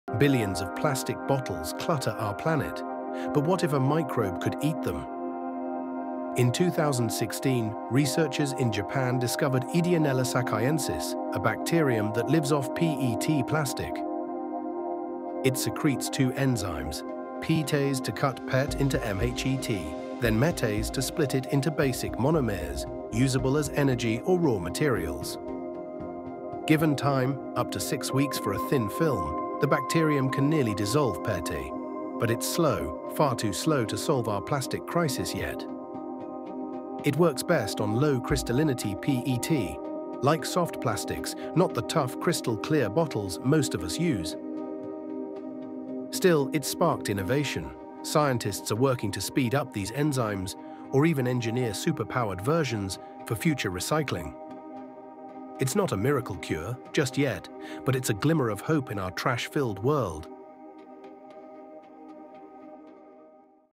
🦠 The Bacterium That Eats Plastic (But Not Fast Enough)
Meet Ideonella sakaiensis, discovered in 2016, which breaks down PET plastic with two enzymes into ethylene glycol and terephthalic acid—the original building blocks. It’s slow and currently impractical for cleanup, but scientists are working on amping it up. Could this microbe one day help us beat the plastic crisis?